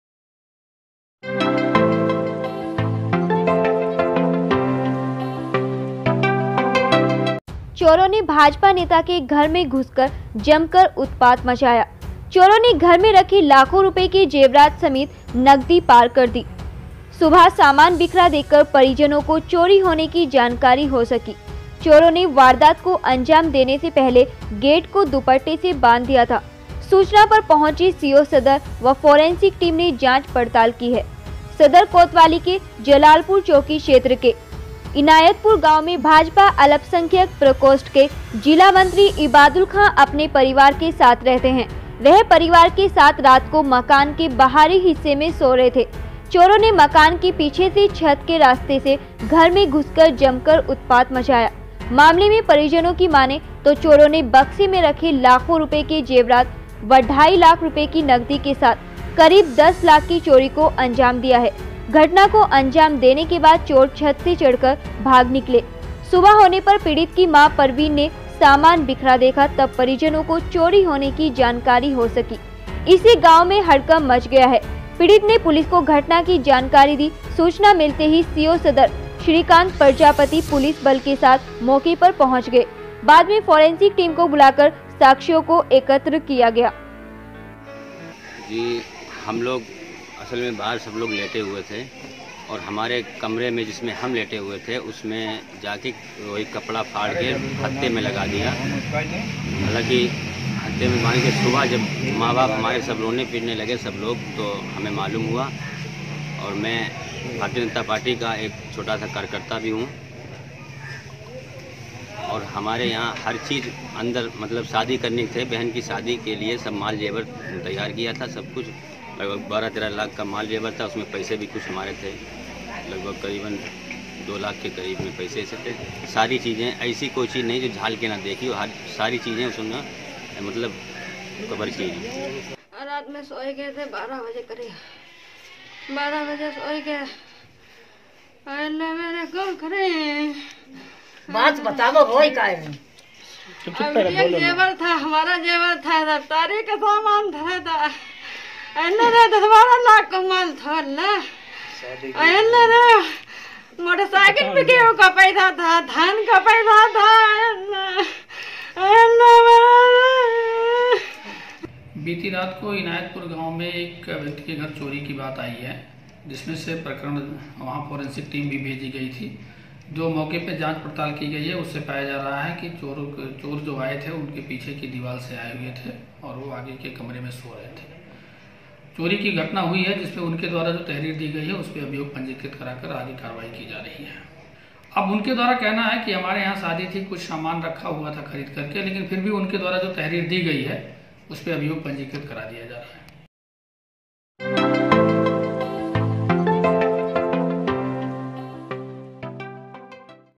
Kannauj: खेती-किसानी करने वाले BJP नेता के घर लाखों की चोरी
कन्नौज में भाजपा नेता के घर हुई चोरी  , लड़की की शादी के लिए रखे जेबरात और लाखों की नकदी लेकर हुए फरार, पुलिस ने किया मामला दर्ज। चोरों ने भाजपा नेता के घर पीछे से घुसकर जमकर उत्पात मचाया। चोरों ने घर में रखे लाखों रुपए के जेवरात समेत नगदी पार कर दी। सुबह सामान बिखरा देख परिजनों को चोरी होने की जानकारी हो सकी। चोरों ने वारदात को अंजाम देने से पहले गेट को दुप्पटे से बांध दिया था। सूचना पर पहुंचे सीओ सदर व फॉरेसिंक टीम ने जांच पड़ताल की।